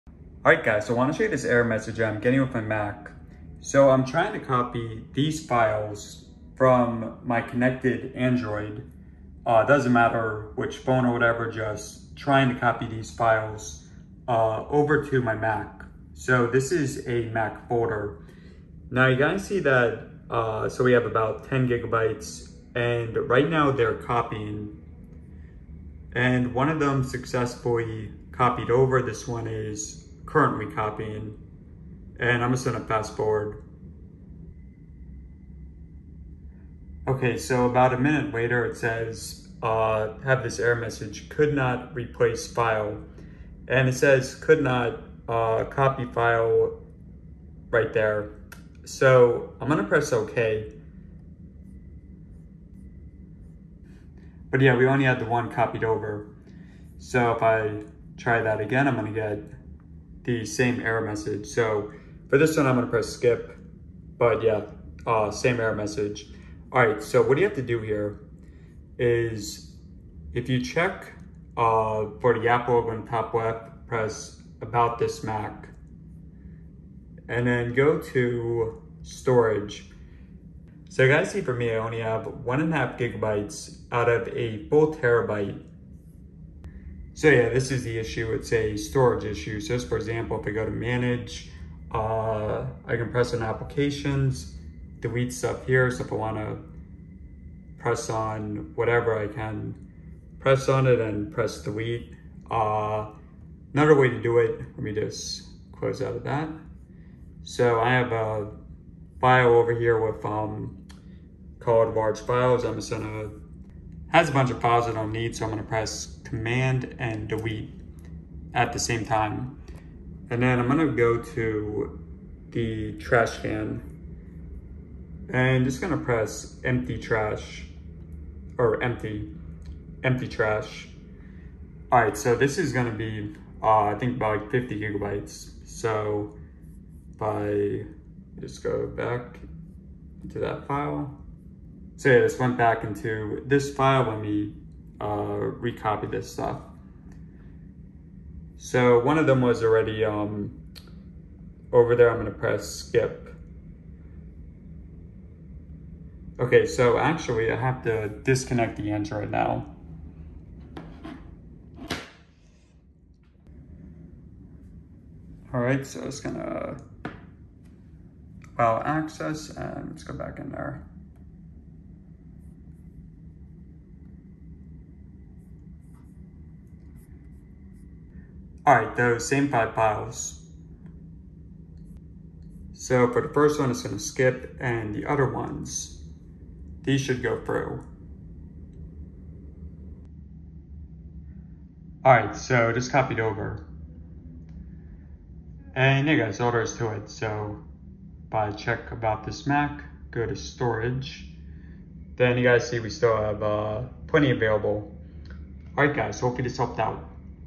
Mac "Could Not Replace File" Error Message Solution
How to fix the Mac error message for "Could Not Replace File", often found with the Android File Transfer. MacBook Pro 16 used.